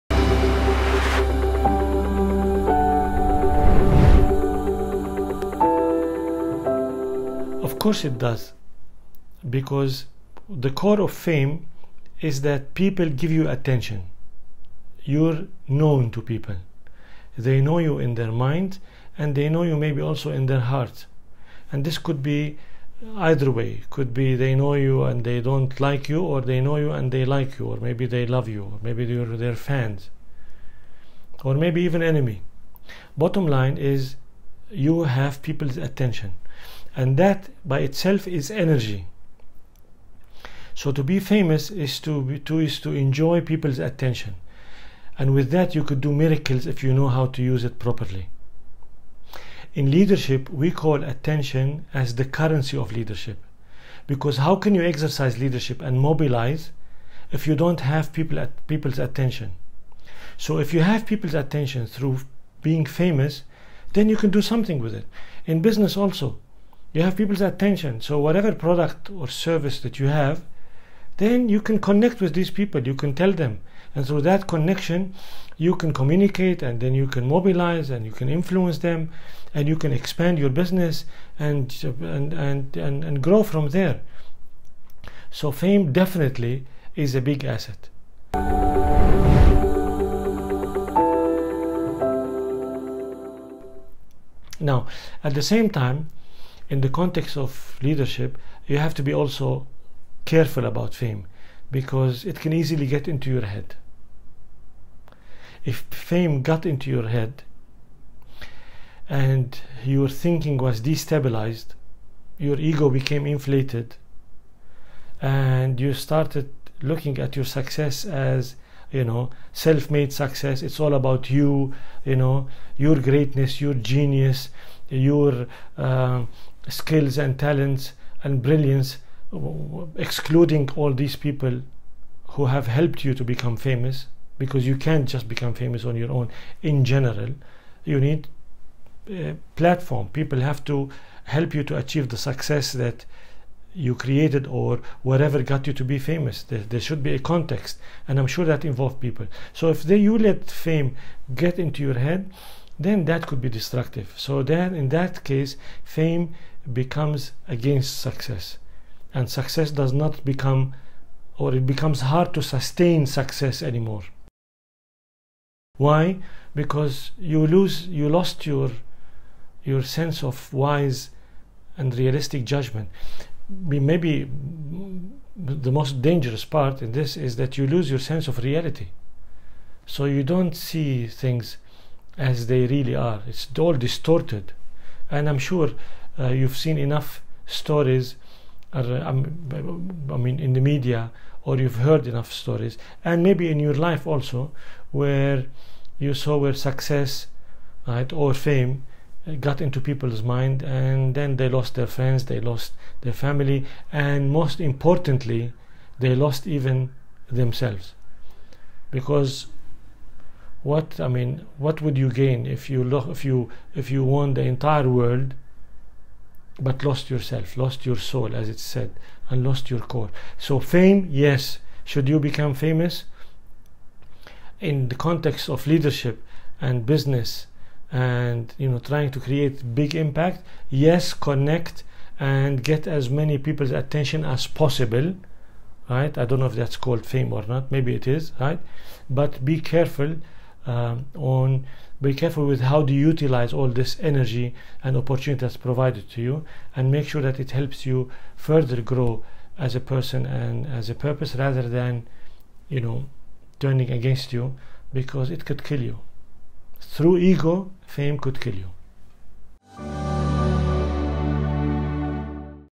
Does Fame Play A Role In Success? | Beyond Leadership
Fame is sought-after by many people especially, with the rise of social media. Bringing fame to improve or distribute the knowledge of your business can work in your favor. Although, when fame is seen as infamy your reputation and business can be affected in a major way.

Beyond Leadership